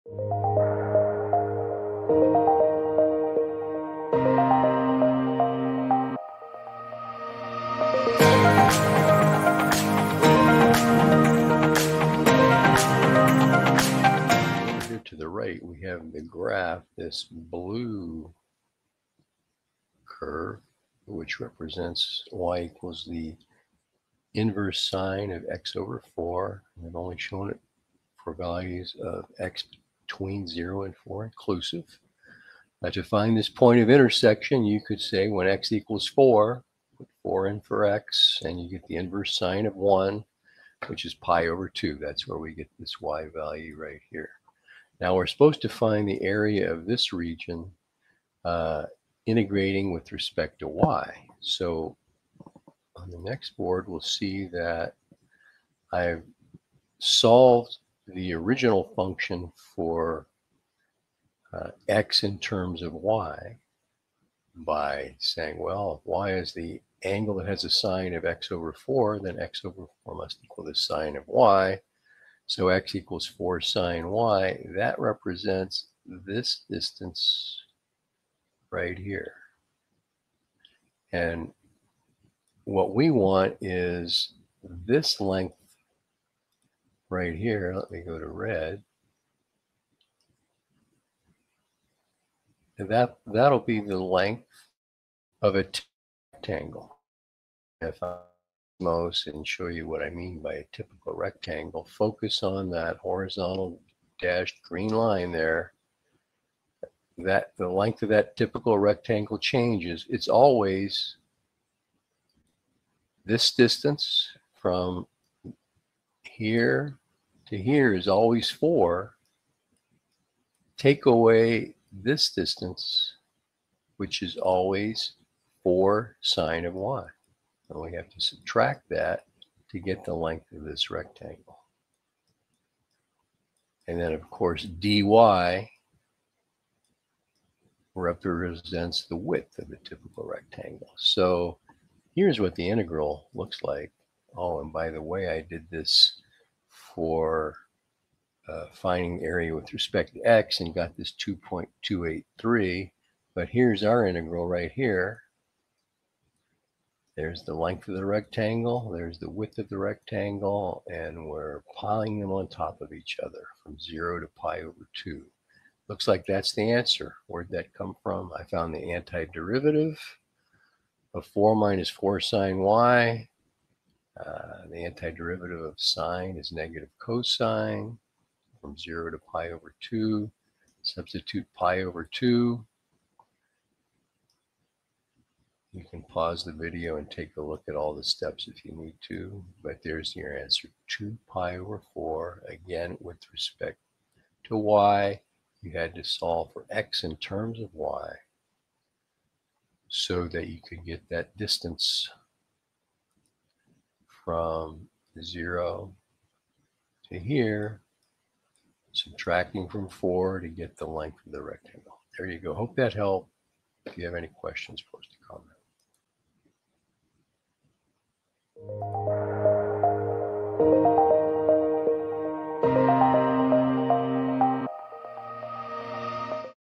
Find area under inverse sing function using definite integral with respect to y.
Question: Find the area of the region bounded by the curves y equals the inverse sine of x divided by 4 , y = 0, and x = 4...
Find the area of the region bounded by the curves  , y = 0, and x = 4 obtained by integrating with respect to y. Your work must include the definite integral and the antiderivative

Answered By:

Doug C.